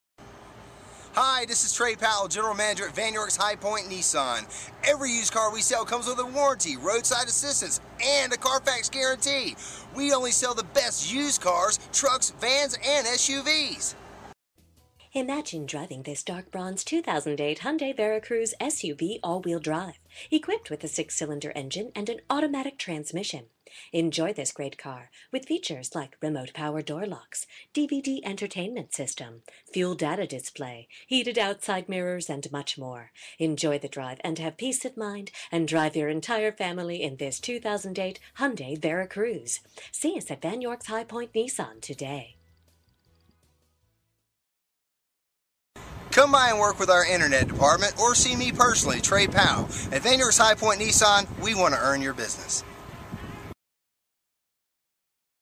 Used 2008 Hyundai Veracruz Limited Greensboro, Winston Salem High Point, Kernersville NC Vann
Used 2008 Hyundai Veracruz Limited Greensboro, Winston Salem High Point, Kernersville NC Vann, Check out this Dark Bronze Hyundai Veracruz SUV AWD, equipped with a 6 Cylinder engine and an automatic transmission. Enjoy this great car with features like Remote Power Door Locks, DVD Entertainment System, Privacy Glass, Fuel Data Display, Heated Outside Mirrors, Rear Air Conditioning, Rear Spoiler, Leather Upholstery, Alloy Wheels, Turn Signal Mirrors, Tire Pressure Monitoring System, Infiniti Stereo System, on steering wheel audio and cruise controls, and much more. Enjoy the drive, have peace of mind, and drive your entire family in this Hyundai Veracruz. See us at Vann York's High Point Nissan today!
MILES: 59,273
VIN:KM8NU73C68U069574

1810 S. Main ST.
High Point, North Carolina 27261